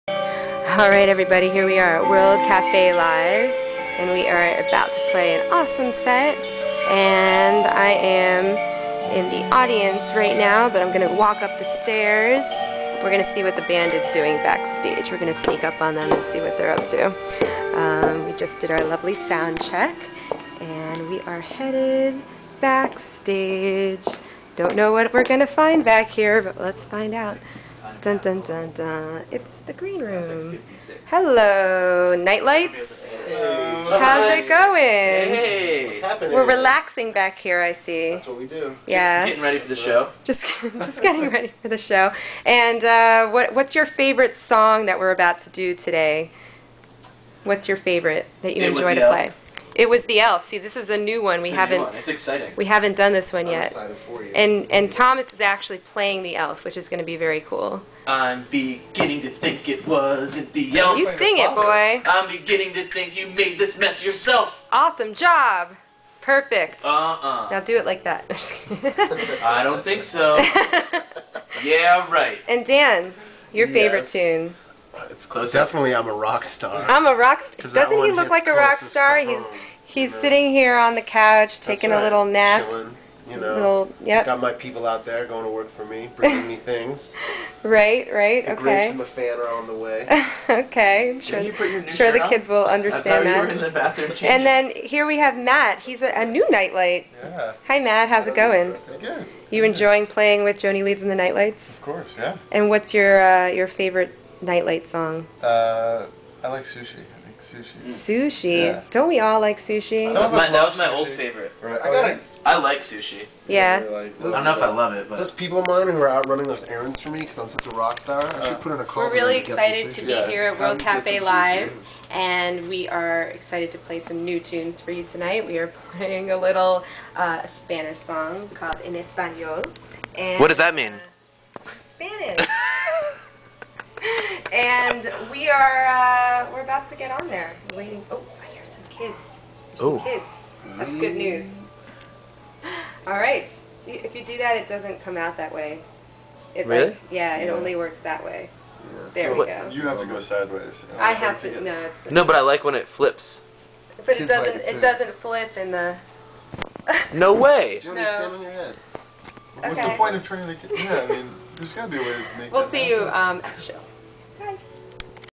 Backstage Silliness at World Cafe Live
Before the World Cafe Live show, Joanie Leeds and the Nightlights relax in the green room after a very early drive.